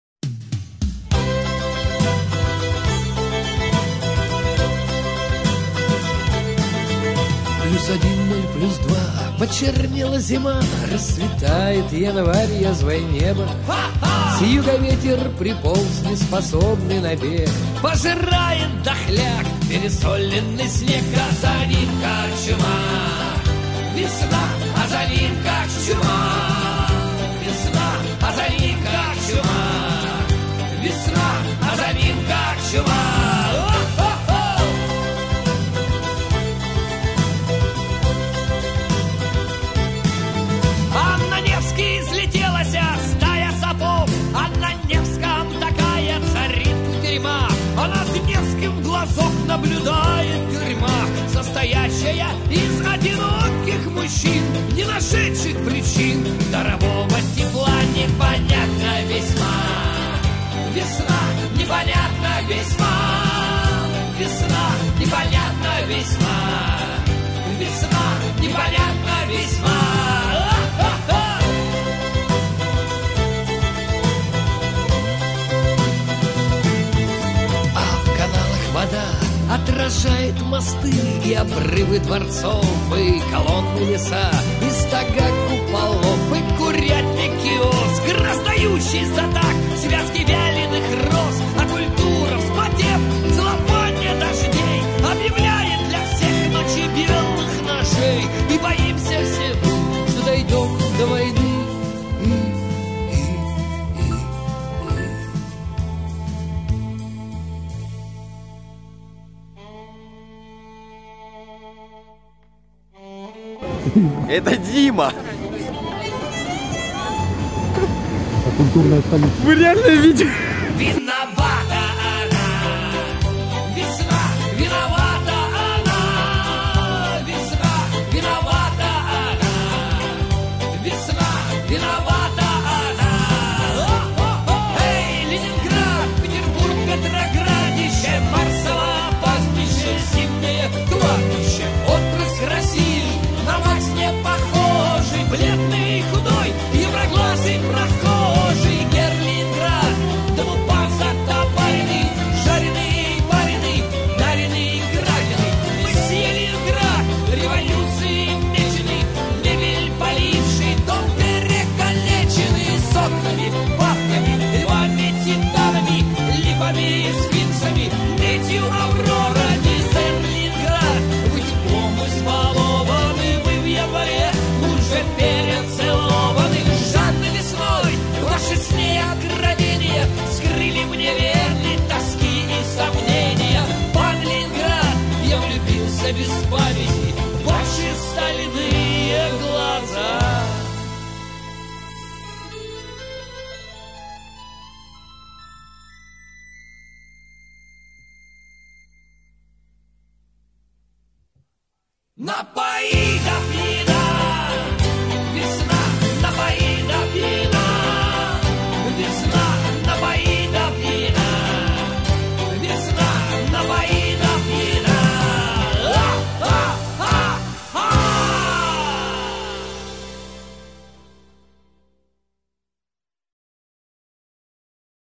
Рождество в Питере. Часть 1
19 студентов СПК провели 5 дней в Петербурге. Видеоотчет прилагается.